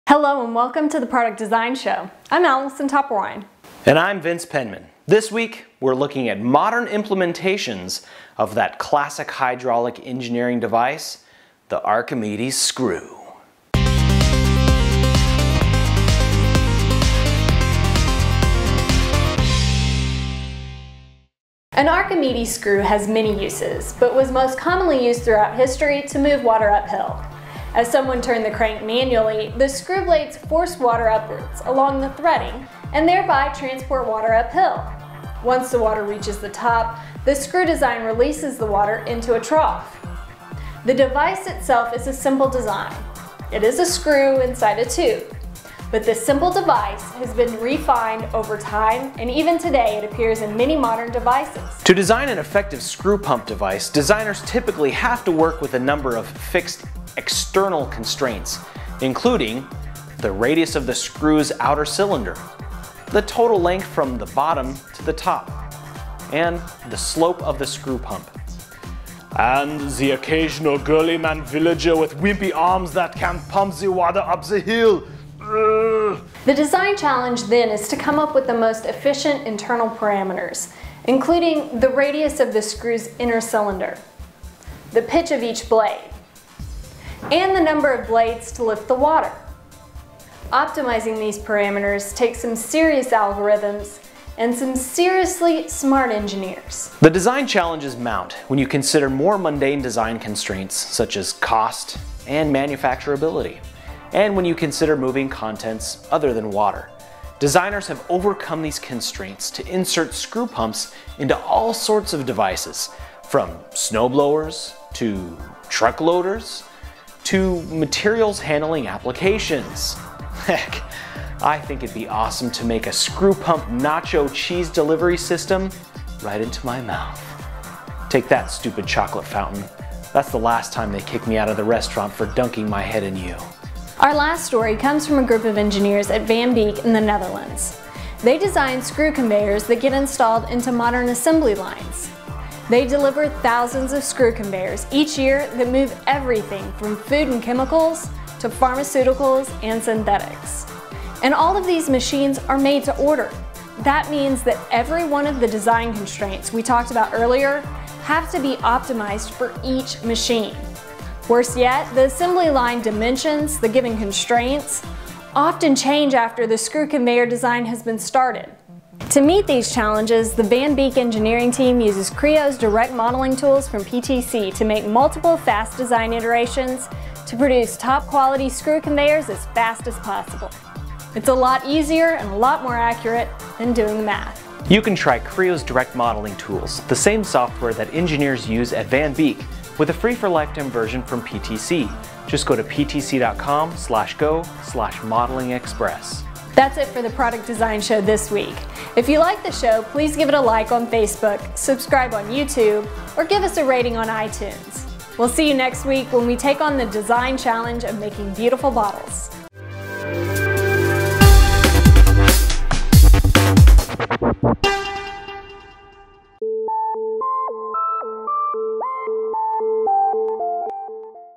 Screw Pump Uses in Modern Equipment
To design an effective screw pump device (Archimedes screw), designers have to work within external constraints, including: i) Radius of the screw's outer cylinder, ii) Total length from the bottom to the top and iii) Slope of the screw pump.

To get your free copy of Creo Elements/Direct Modeling Personal Edition 3.0, go